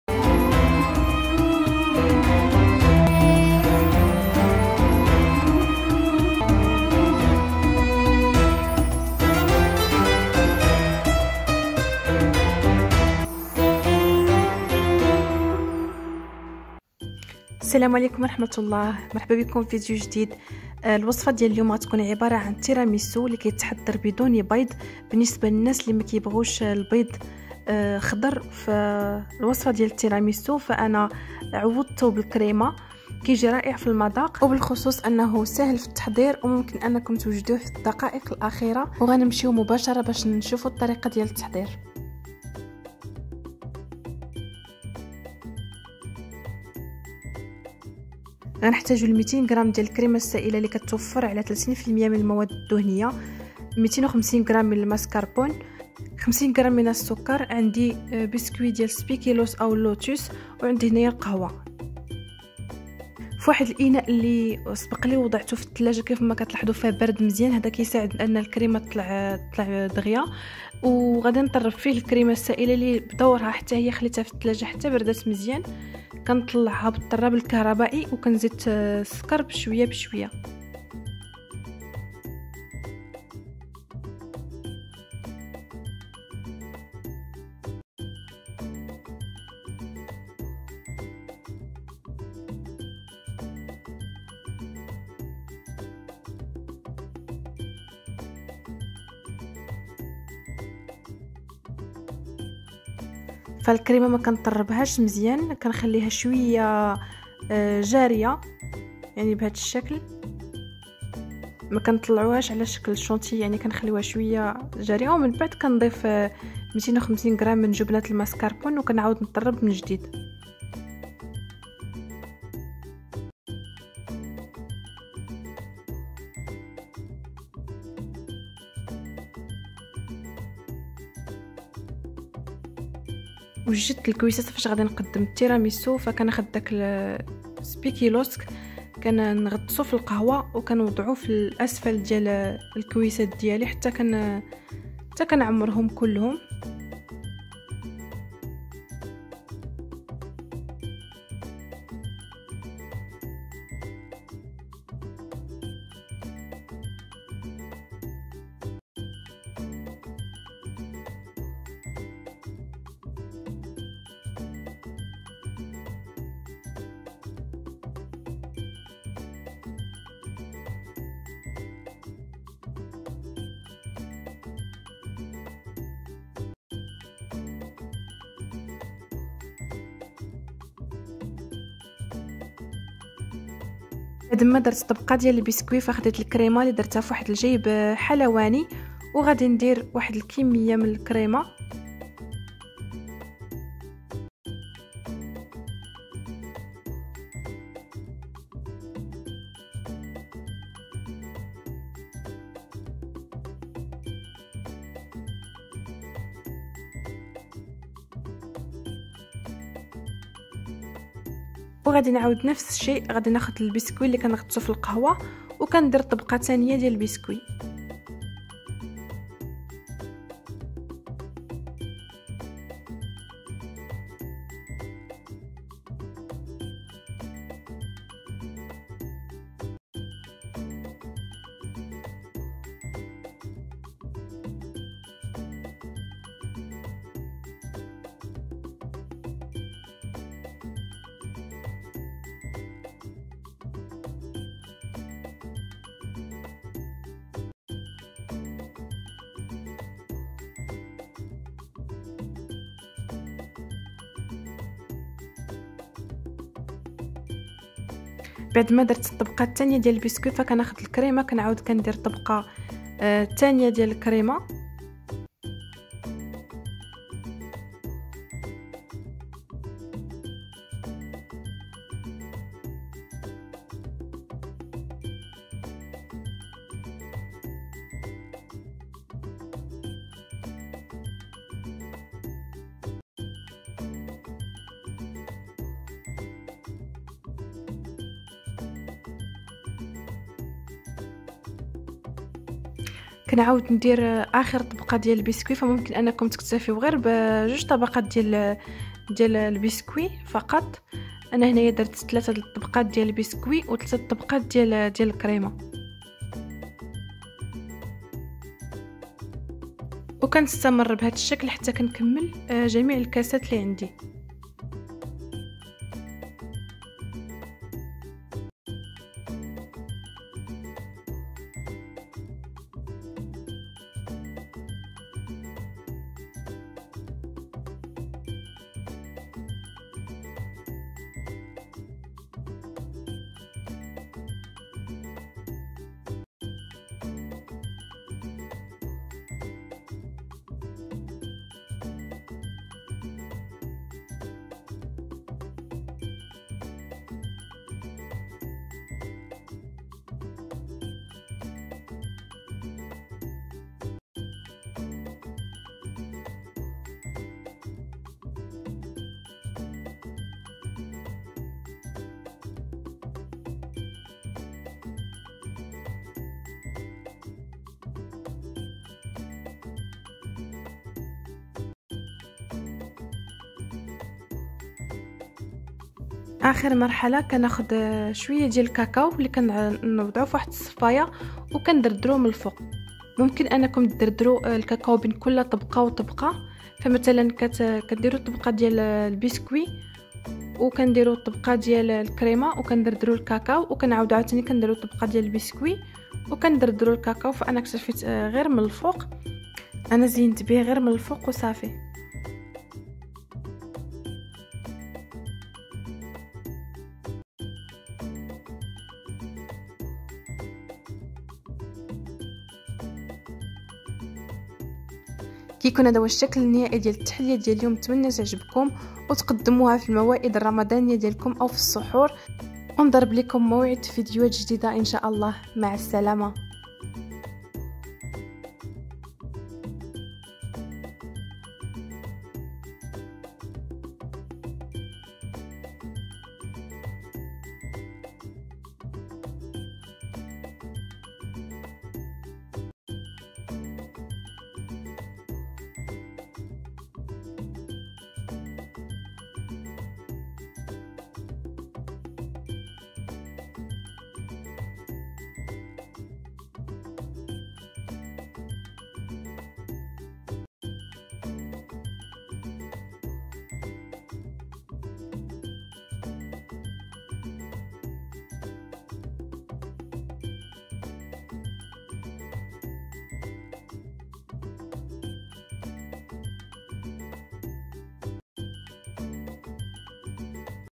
تحلية التيراميسو باللوتوس بدون بيض حضريها في دقائق معدودة
المقادير:
200 gكريمة سائلة
250 g جبنة الماسكربون
50gسكر
قهوة
بسكوي اللوتوس